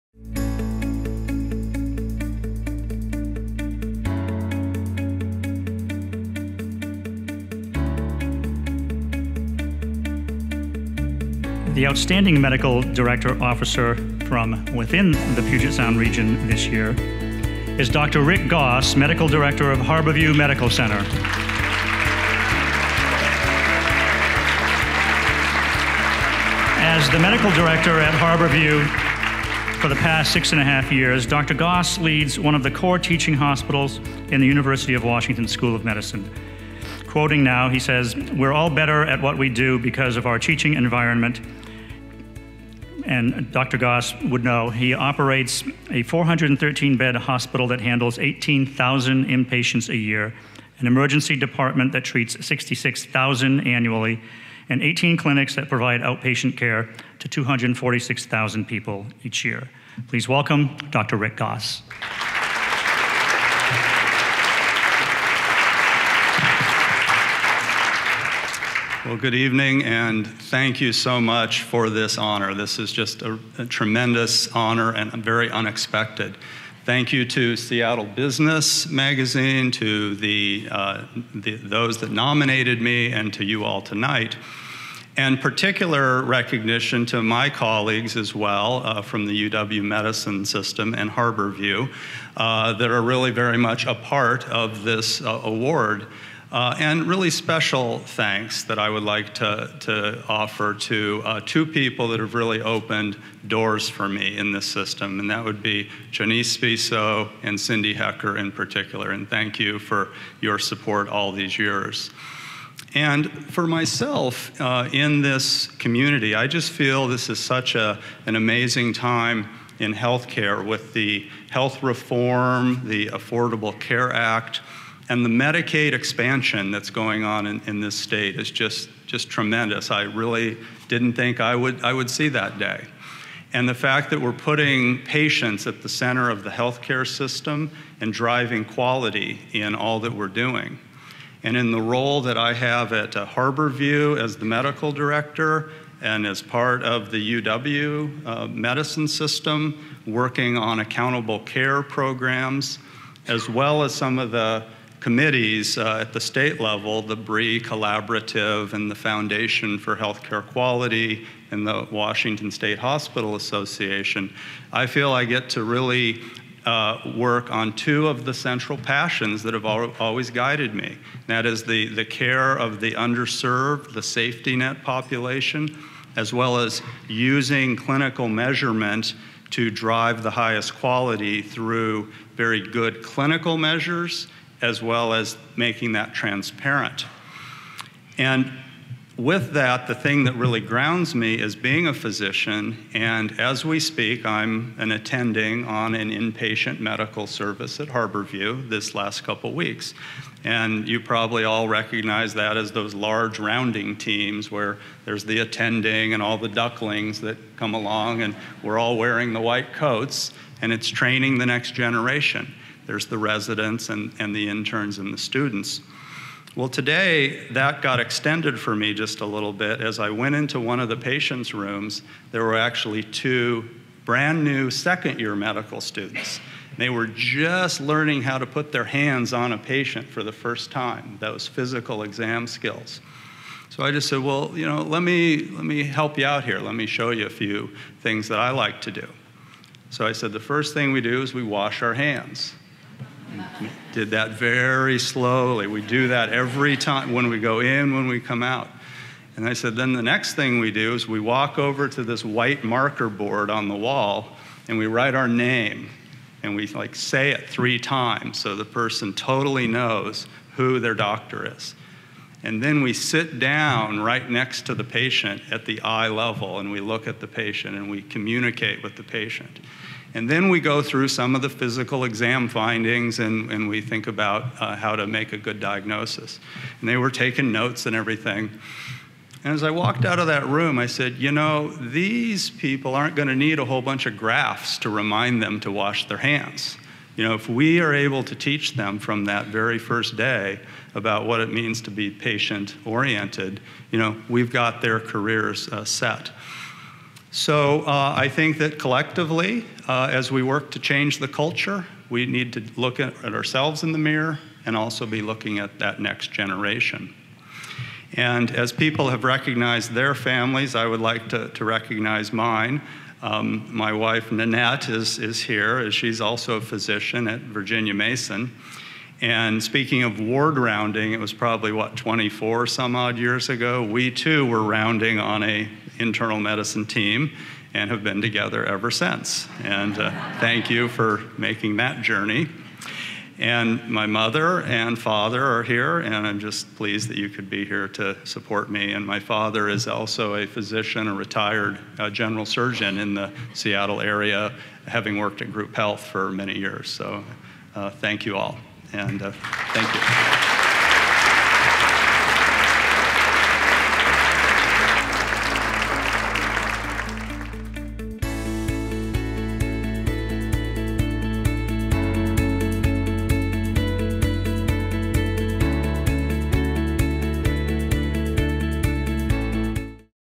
Leaders in Health Care Awards 2015: J. Richard Goss, M.D.
J. Richard Goss, M.D., Medical Director of Harborview Medical Center accepts the 2015 Leaders in Health Care Awards for Outstanding Medical Director in the Puget Sound Region.